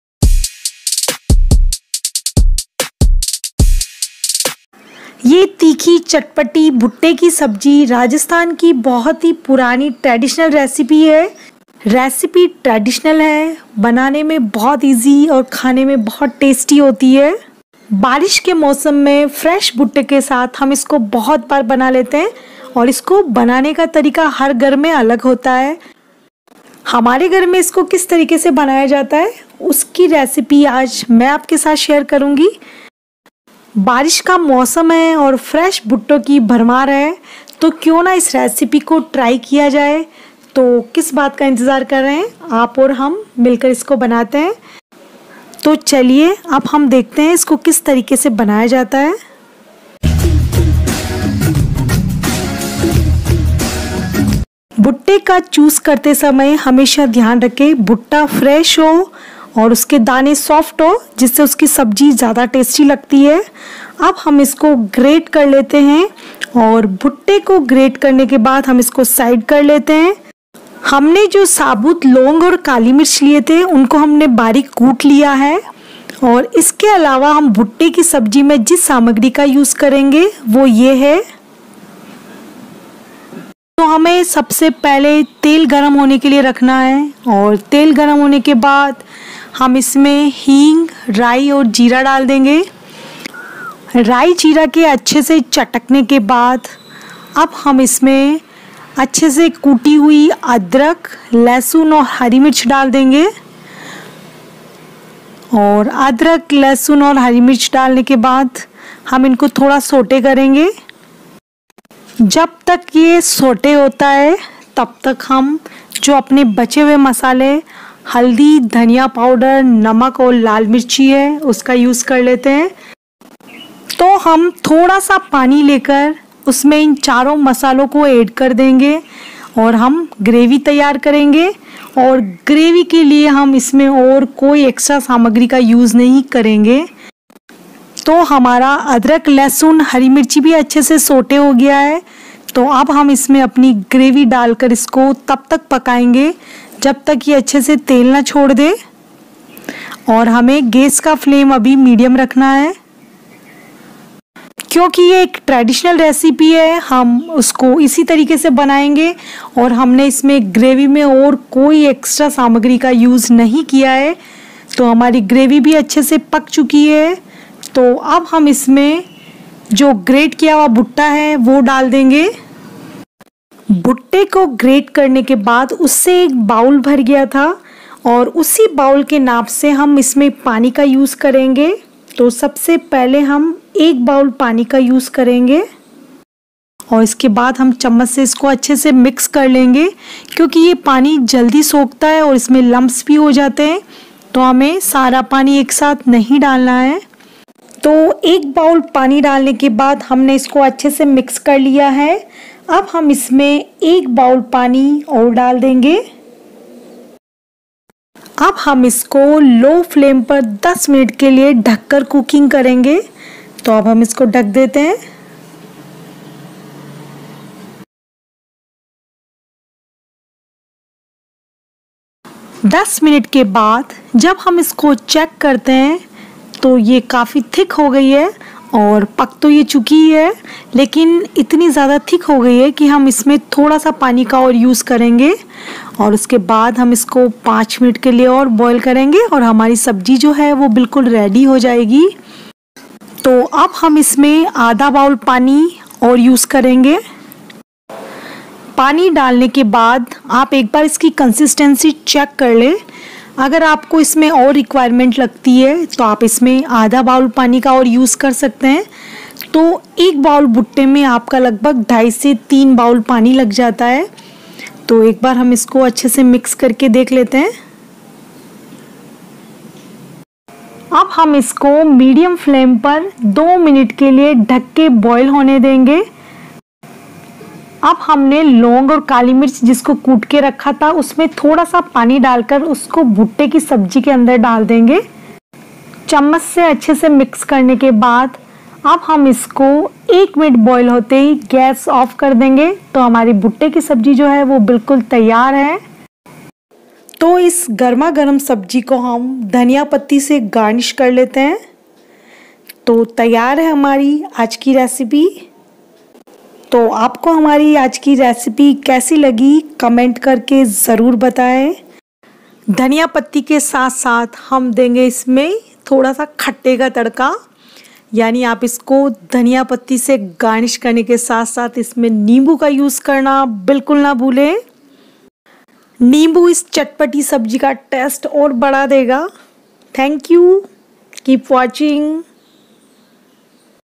भुट्टे की सब्जी||rainy season specialभुट्टे की सब्जी बनाने का अब तक का सबसे सरल और पारंपरिक तरीका
Hello Foodies

In today's video, I have told you the simple and traditional way to make corn vegetable
We always miss the traditional recipe
So let's try this traditional recipe together with you
So you try this recipe and send me your feedback

Please give your comment and suggestions in the comment section below
Hope u like Today's recipie.